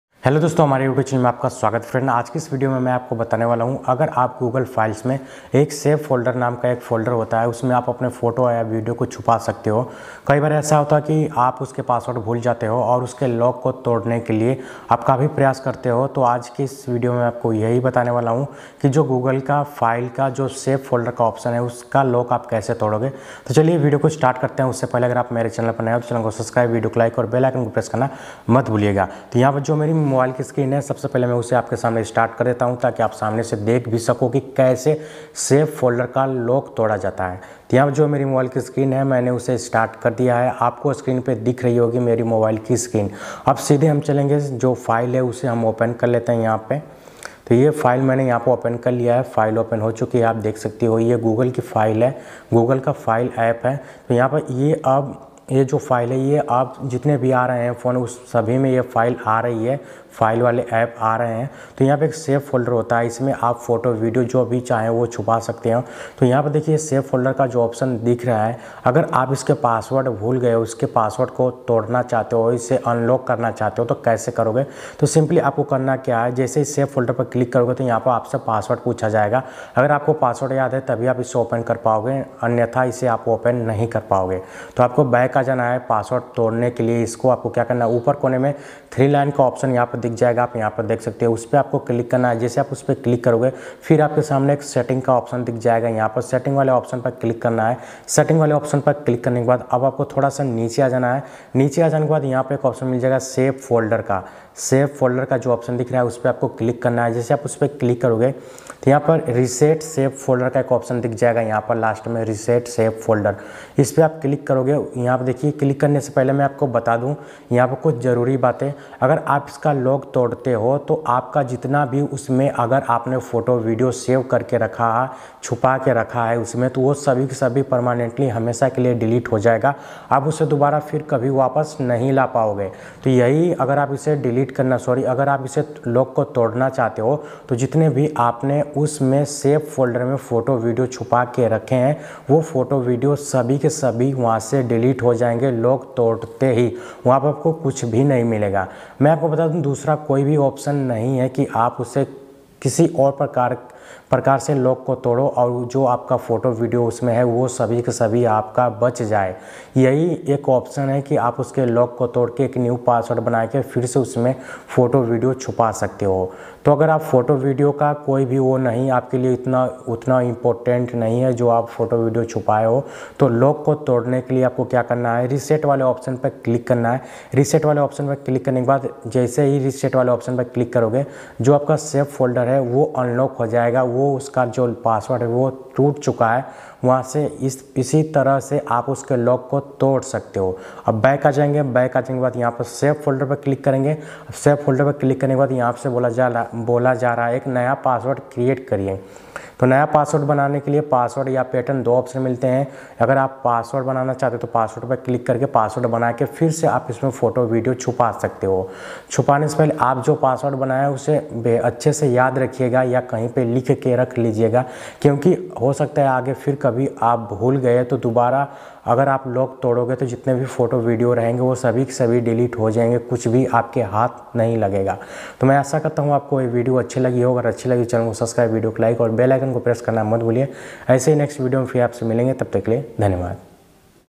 Safe folder ka lock kaise tode | safe folder ka password bhul gaye to kaise tode
Safe folder ka lock kaise tode
Safe folder ka password bhul gaye to kaise tode

Hi dosto aaj ki es video me maine aapko bataya hai ki safe folder ka lock kaise tode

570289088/